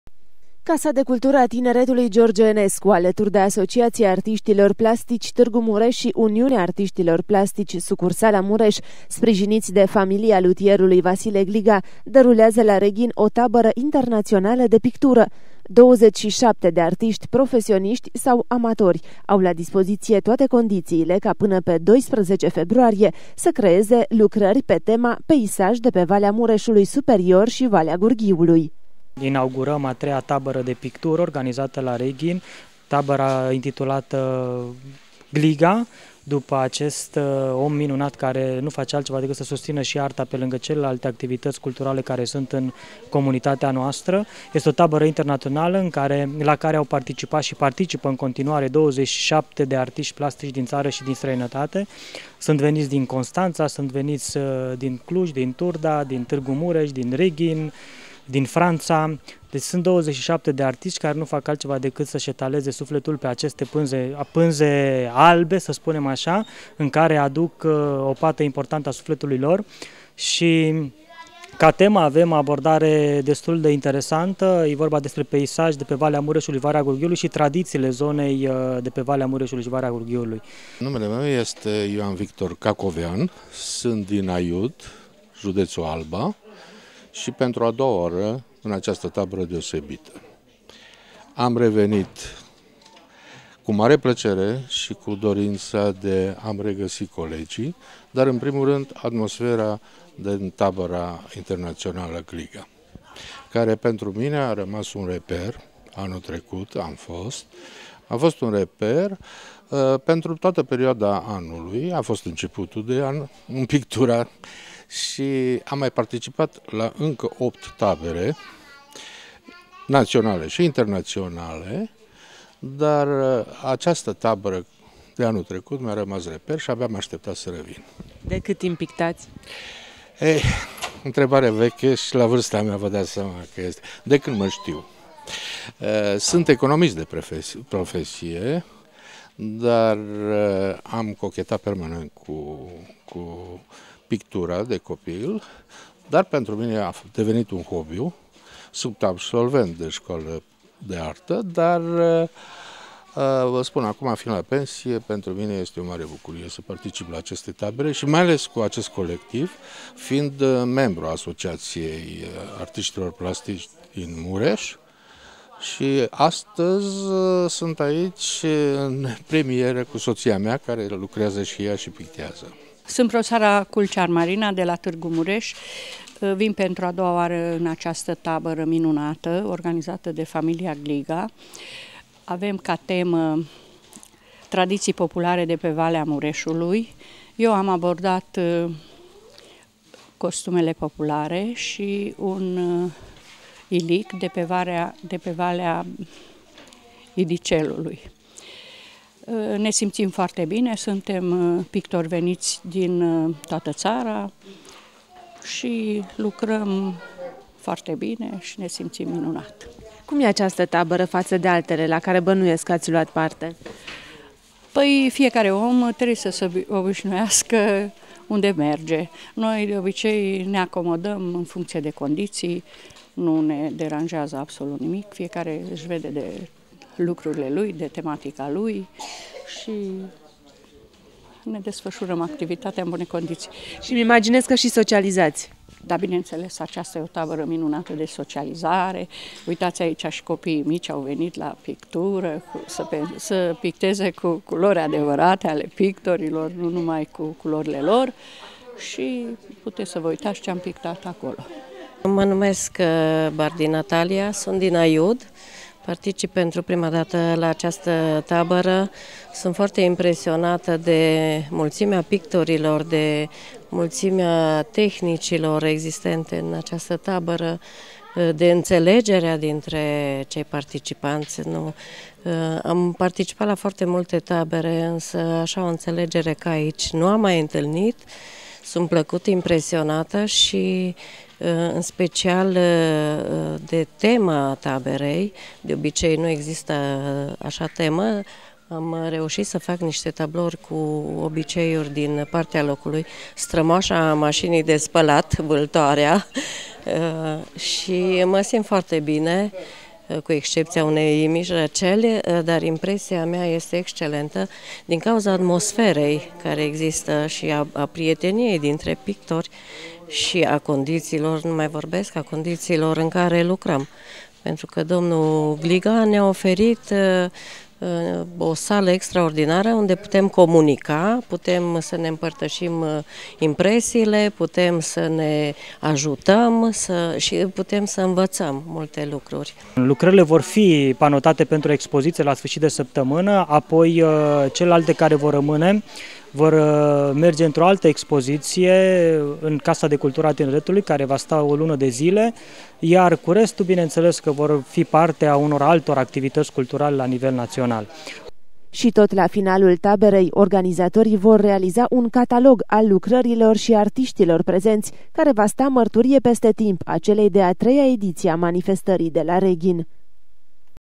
TABĂRĂ INTERNAŢIONALĂ DE PICTURĂ, LA REGHIN (2017 02 08)
Peisaj de pe Valea Mureșului Superior și Valea Gurghiului. Aceasta este tema celei de a treia ediţii a taberei internaţionale de pictură de la Reghin. Manifestarea are loc la sala de evenimente de pe strada Pandurilor în organizarea Asociaţiei şi Uniunii Artiştilor Plastici Mureş, a Casei de Cultură „G. Enescu” şi a familiei lutierului Vasile Gliga. Participă 27 de pictori care rămân la Reghin până pe 12 februarie.